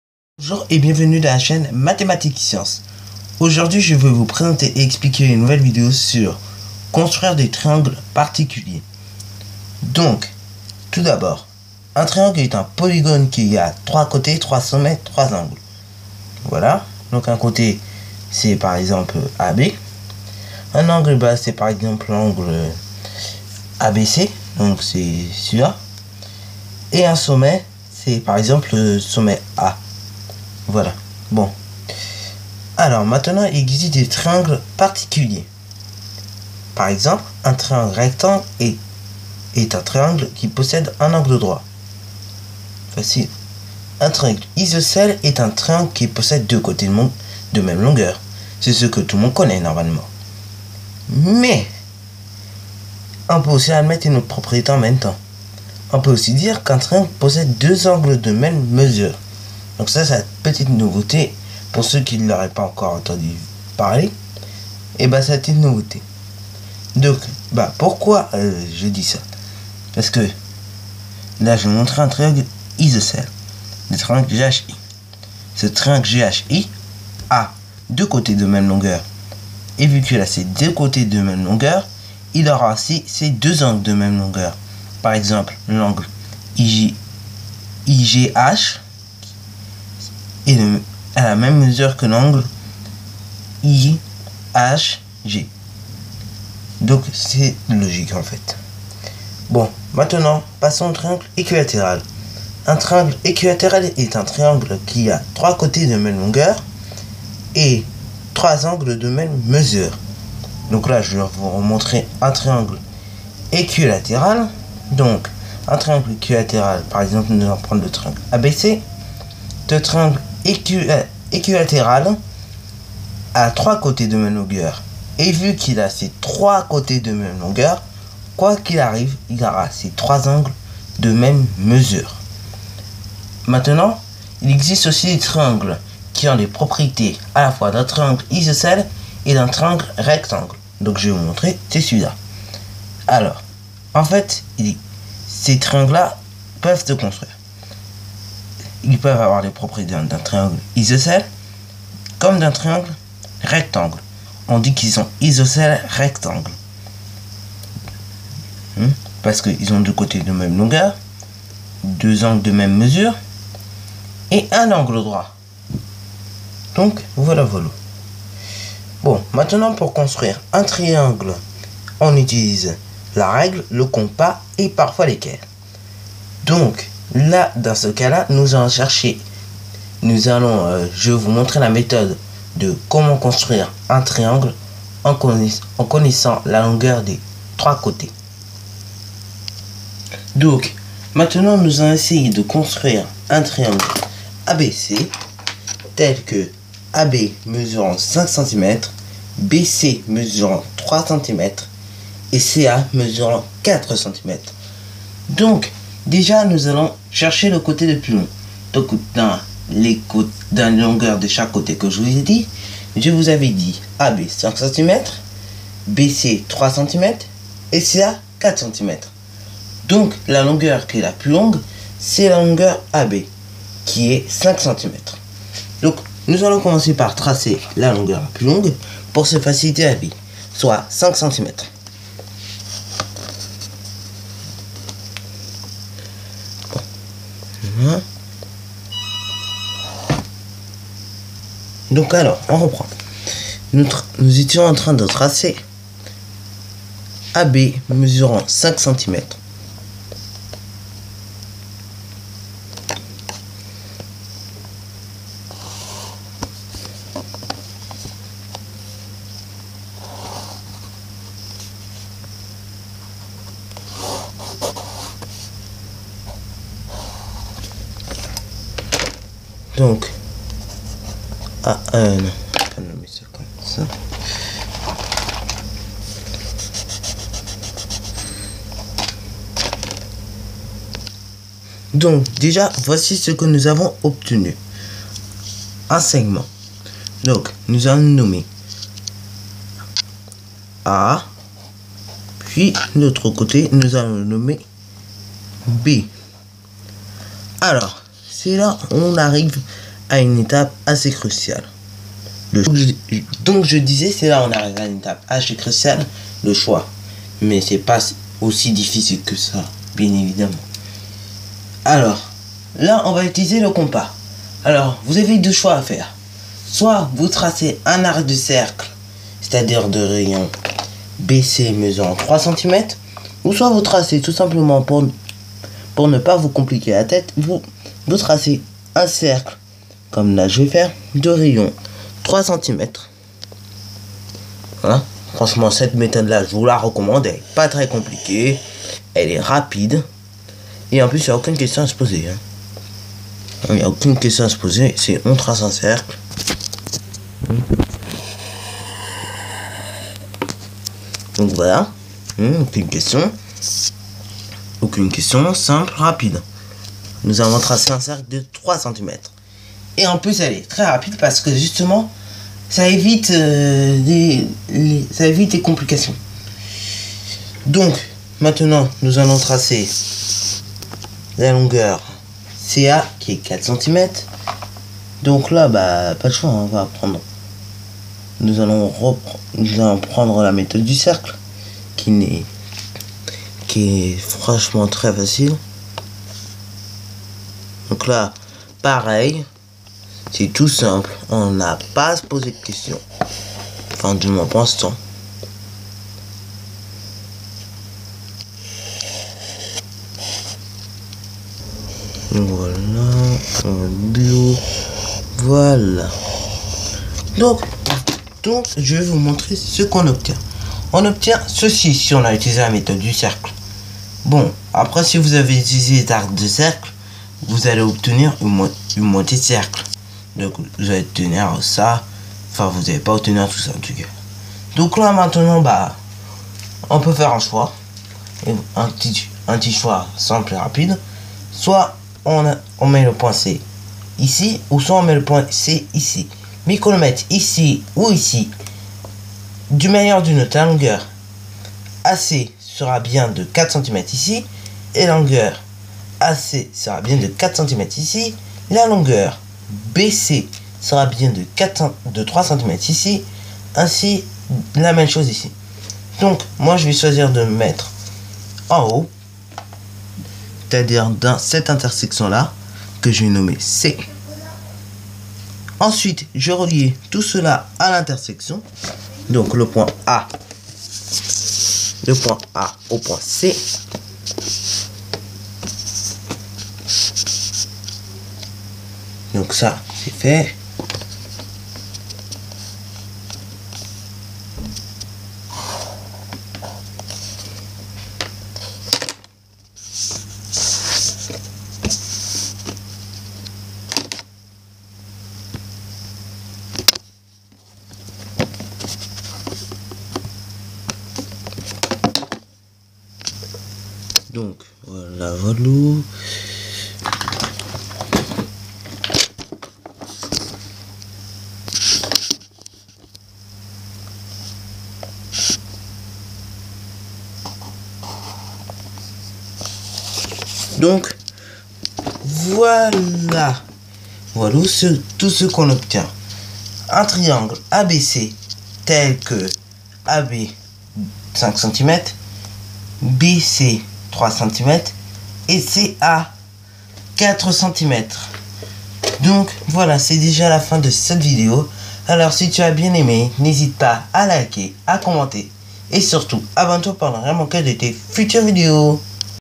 Espace et géométrie : Construire des triangles particuliers
Voici une nouvelle vidéo sur la construction de triangles ( quelconques et particuliers ).
⚠Information: la vidéo que j'avais promise de faire sur la construction de quadrilatères particuliers et sur les triangles particuliers se fera après la vidéo: Construire une figure selon un programme de construction. Et cette vidéo se fera en 2 thèmes:
Thème 1: Construire des quadrilatères particuliers (carrés, losanges,  rectangles)
                   et des quadrilatères (parallélogrammes).
Thème 2: Construire des triangles particuliers ( isocèles, équilatéral, rectangle)